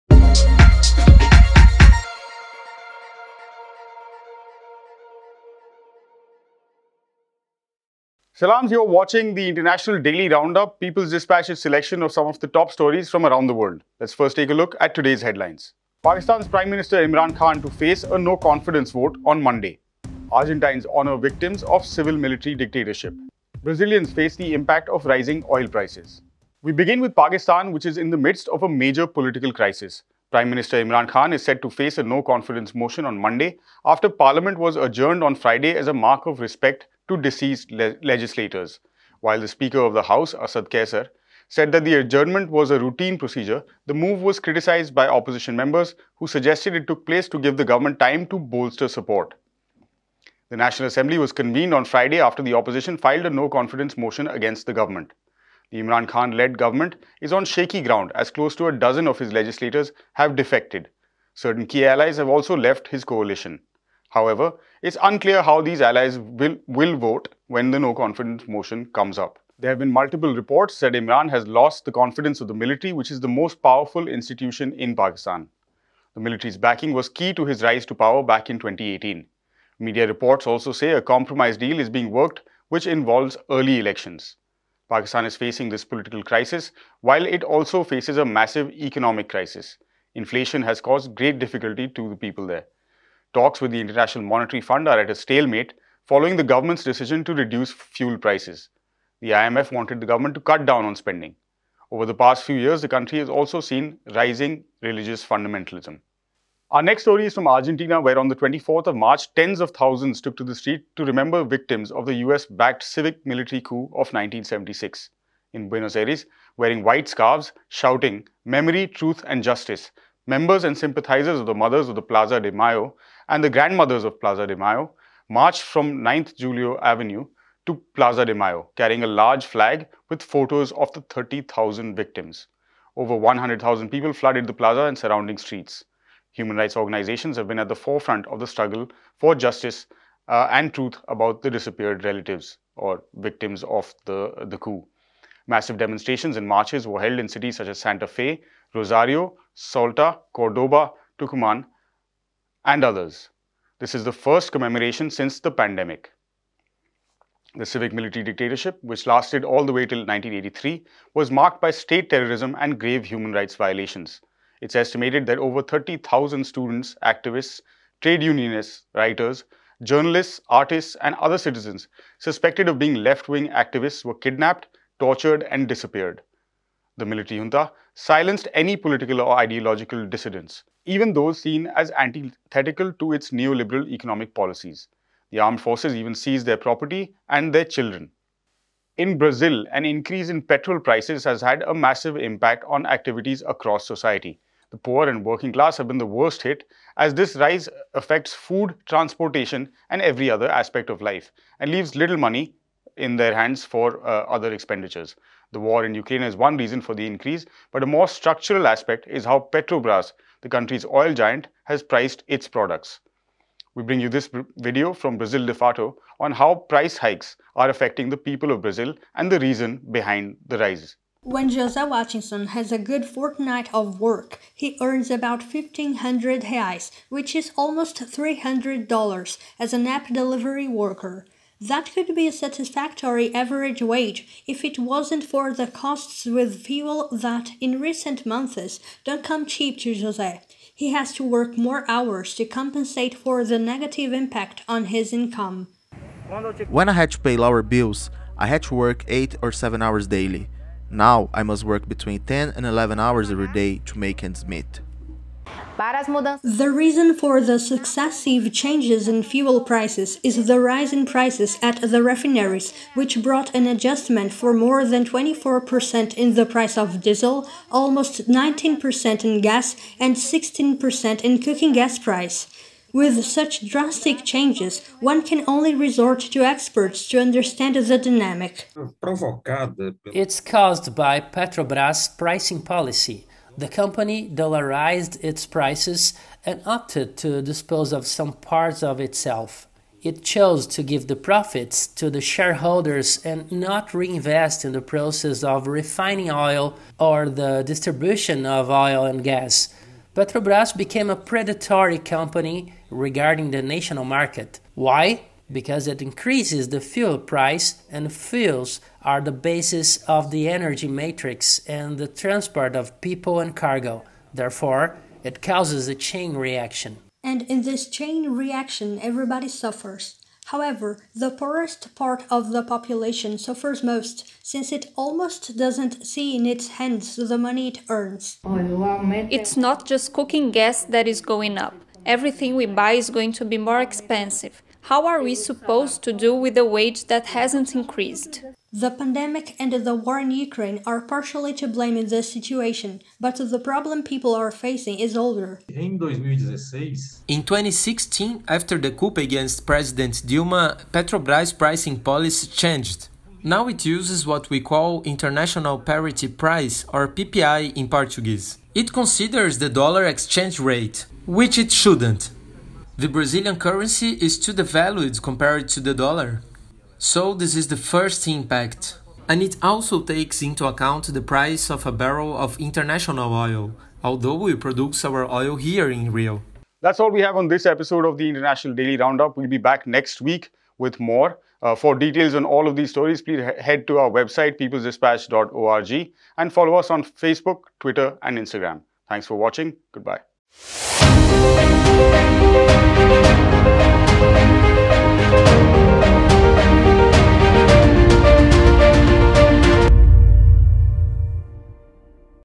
Daily Round up | Political crisis in Pakistan, Argentines honor coup victims and other stories
In today’s episode of the Daily Round-up, we look at the adjournment of Pakistan's National Assembly where prime minister Imran Khan is facing a no-confidence motion. We also look at the marches by the people of Argentina to commemorate the victims of the civil-military coup, and the struggles of people in Brazil due to high fuel prices.